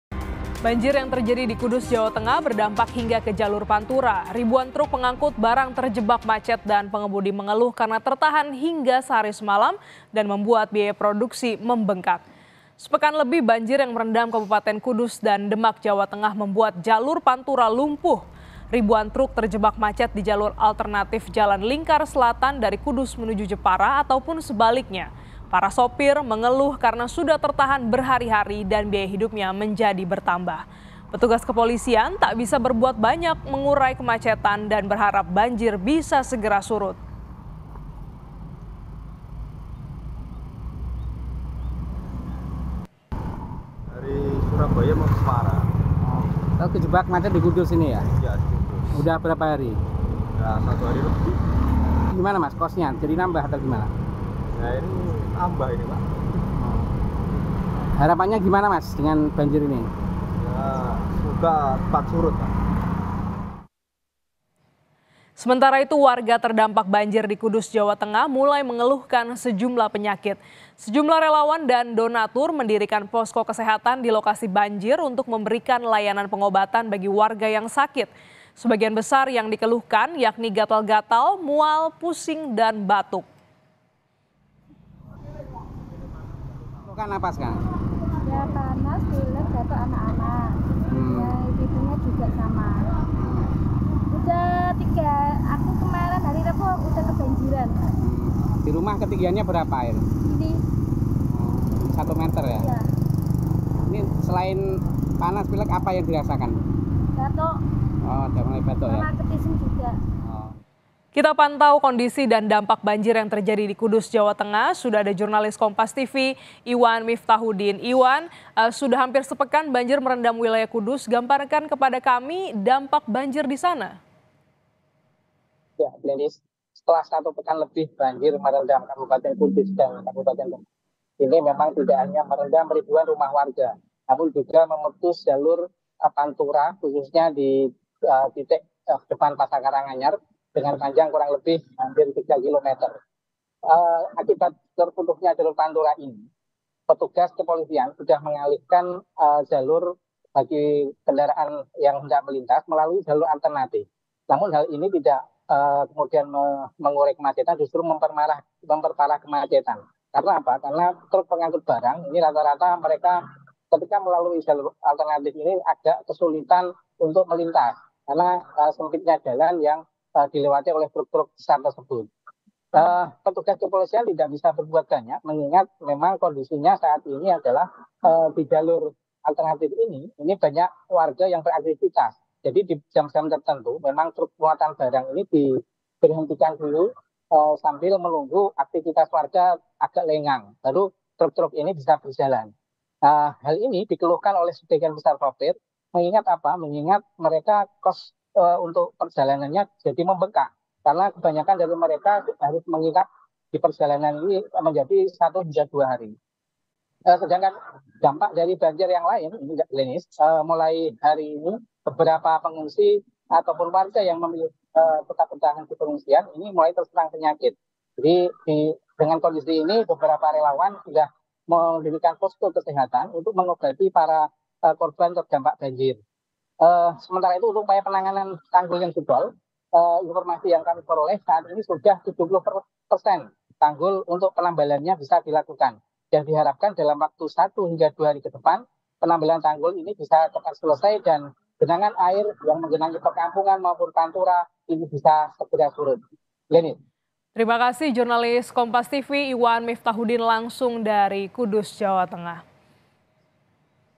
Dampak Banjir Kudus Jalur Pantura Kudus Jepara Macet, Warga Keluhkan Sakit
KOMPAS.TV - Banjir yang terjadi di Kudus, Jawa Tengah berdampak hingga ke Jalur Pantura.

Ribuan truk pengangkut barang terjebak macet dan pengemudi mengeluh karena tertahan hingga sehari semalam dan membuat biaya produksi membengkak.

Sepekan lebih banjir yang merendam Kabupaten Kudus dan Demak, Jawa Tengah membuat jalur pantura lumpuh.

Ribuan truk terjebak macet di jalur alternatif jalan lingkar selatan dari Kudus menuju ke Jepara ataupun sebaliknya, para sopir mengeluh karena sudah tertahan berhari-hari dan biaya hidupnya menjadi bertambah.

Petugas kepolisian tak bisa berbuat banyak mengurai kemacetan dan berharap banjir bisa segera surut.

Sementara itu, warga terdampak banjir di Kudus, Jawa Tengah mulai mengeluhkan sejumlah penyakit.

Sejumlah relawan dan donatur, mendirikan posko kesehatan di lokasi banjir untuk memberikan layanan pengobatan bagi warga yang sakit.

Sebagian besar yang dikeluhkan yakni gatal-gatal, mual, pusing dan batuk.